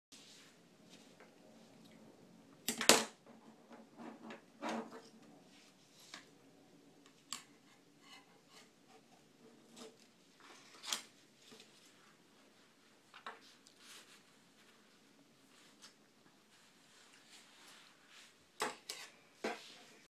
Harry using scissors for the first time
Using the new safety scissors he got for his birthday